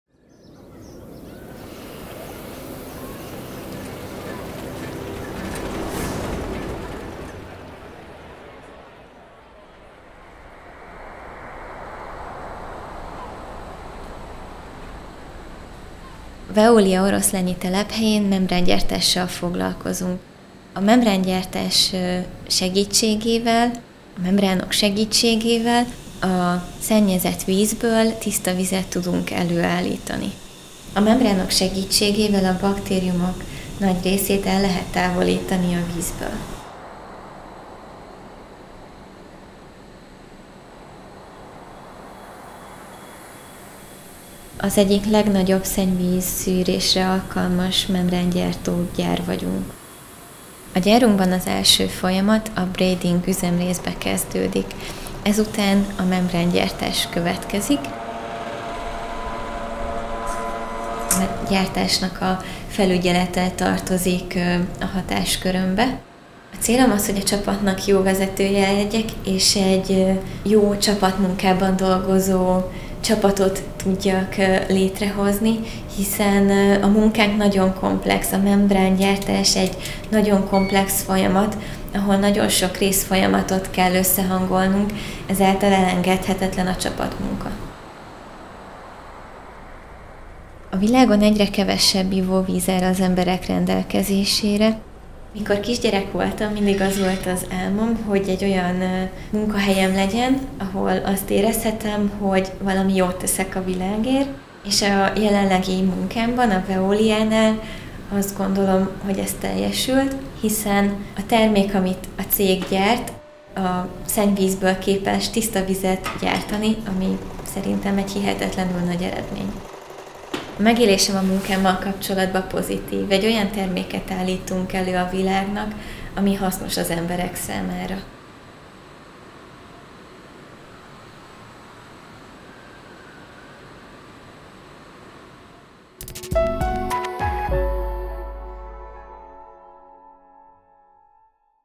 Find out how Dorottya's job, our Hungarian resourcer, contributes to clean up water | Veolia
Come with us and meet the people who are optimistic about ecological transformation.
At the Oroszlány site in Hungary, meet Dorottya, Production Supervision Team Leader, who explains the solutions to mass produce water filtration membranes. With this innovative technology, it is possible to treat all types of water 💧 in an optimal and sustainable way 🌍♻️.

00:19 What are membranes used for?
00:33 How do membranes work ?
00:46 The world's largest membrane production plant
01:05 My role is to supervise production and coordinate teams
01:20 The manufacturing process
01:54 My childhood dream has come true with Veolia: to act for the environment and to be useful to turn the tide!

Want to learn more about our Veolia's solutions:
👉 How do we fight water pollution?

If, like her, you want to act for the Ecological Transformation, join us!

Découvrez comment le travail de Dorottya, notre ressourceur hongroise, contribue  à la dépollution de l'eau | Veolia

Venez avec nous à la découverte de celles et ceux qui sont déterminés pour la transformation écologique.
En Hongrie, rencontrez Dorottya, cheffe d'équipe - supervision de la production sur le site d'Oroszlány qui nous explique les solutions déployées pour produire massivement des membranes de filtration d'eau. Grâce à cette technologie novatrice, il est possible de purifier de façon optimale et durable tous les types d’eau 💧🌍♻️.

00:19 A quoi servent les membranes?
00:33 Comment fonctionnent les membranes ?
00:46 La plus grande usine de production de membranes au monde
01:05 Mon rôle consiste à superviser la production et coordonner les équipes
01:20 Processus de fabrication
01:54 Mon rêve d’enfant s’est réalisé avec Veolia, agir pour l’environnement et être utile pour changer la donne !

Envie d'en savoir plus sur les solutions de Veolia :
👉Comment lutter contre la pollution de l’eau ?

Si comme elle, vous voulez agir pour la transformation écologique, rejoignez-nous !